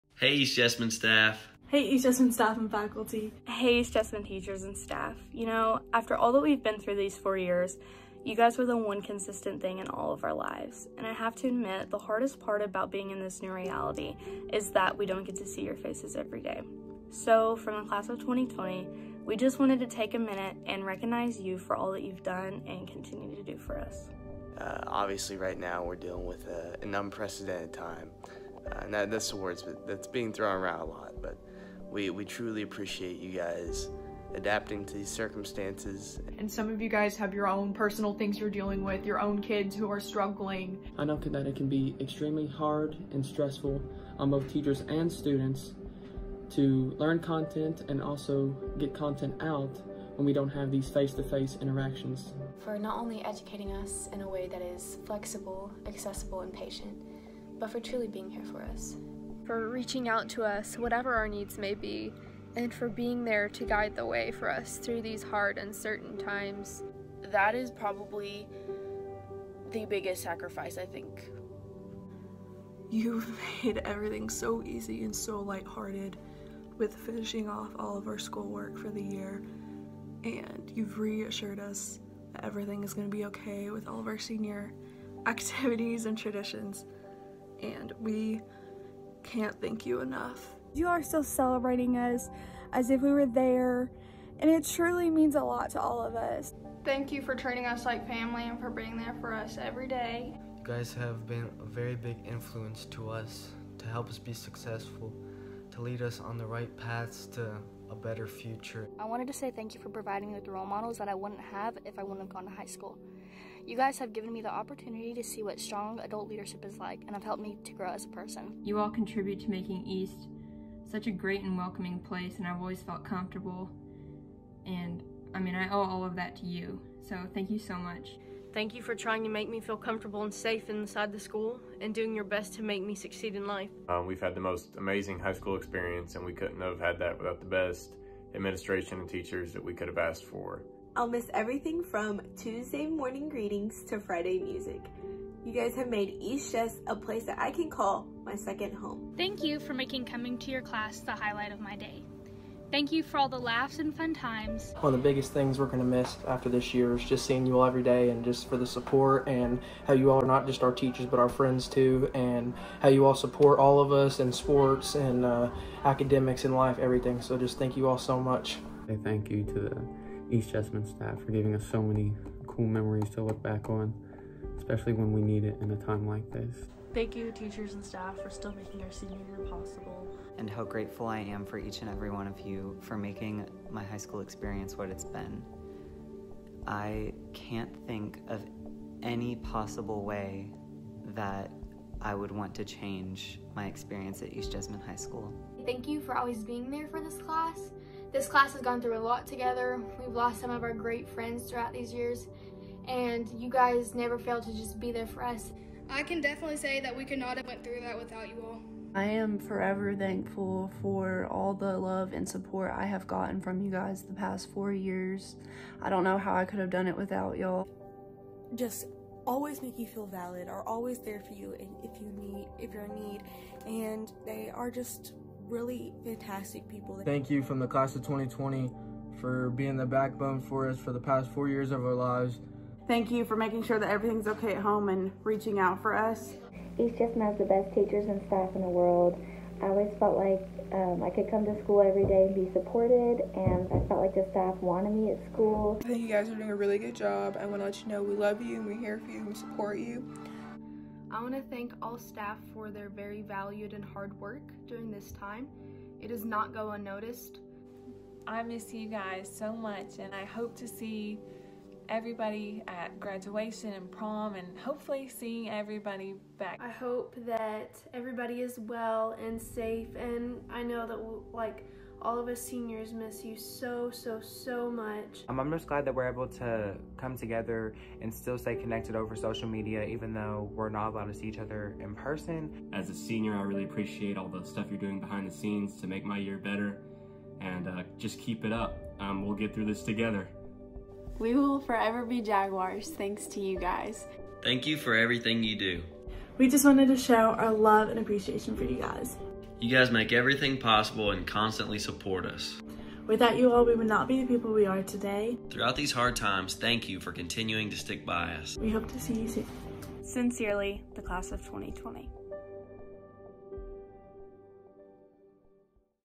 Thank You
East Jessamine High School Class of 2020 expresses their gratitude to the teachers and staff who have walked by them the past four years, especially throughout the COVID-19 pandemic.